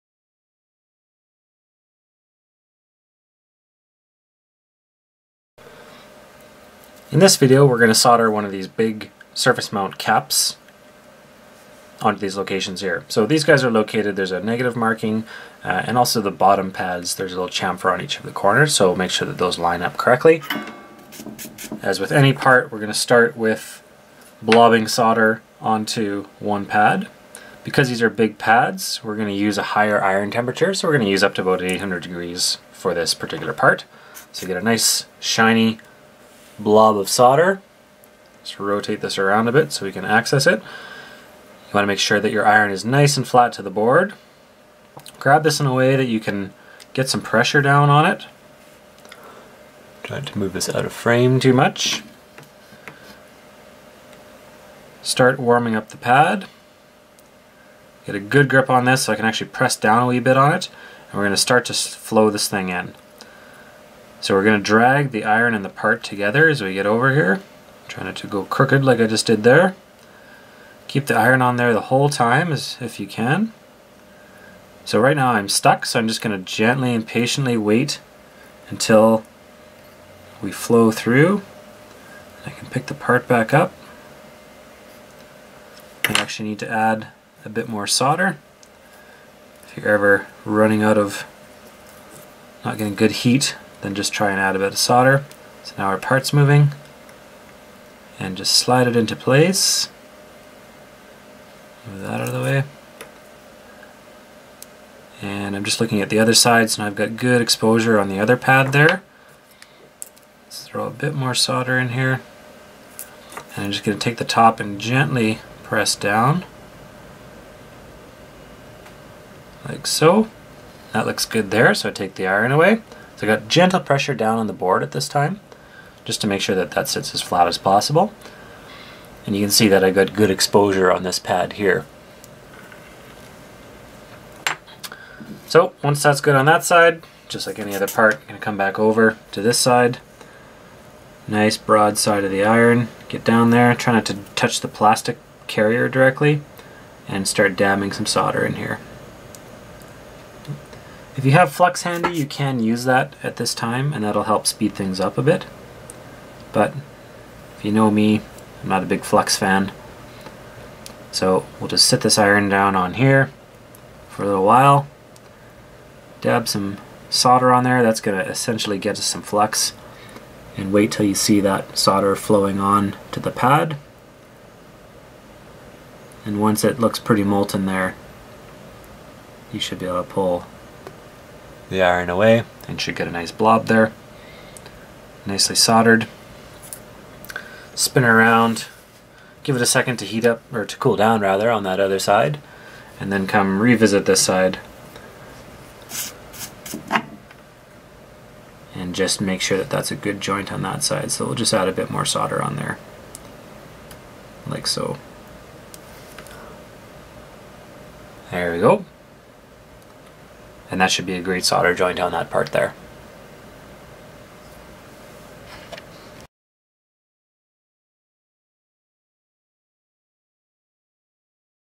Soldering a large SMT Capacitor
Here is a video showing how to solder the big electrolytic caps on the MPG development boards.

Note: we are well aware this is turned out to be BRUTAL, but chose to keep the whole video because it shows a nice recovery.  Occasionally stuff like this happens but as a hobbiest you don't have the luxury of tossing out the board.  We have seen people totally destroy PCBs when something goes wrong, and wanted to show how to patiently and carefully recover.  The board is NOT too hot because this cap is on a massive heat sink -- if the solder isn't melting, it's not hot.  If we were attempting to show IPC grade soldering, this video would have been in the trash long, long ago.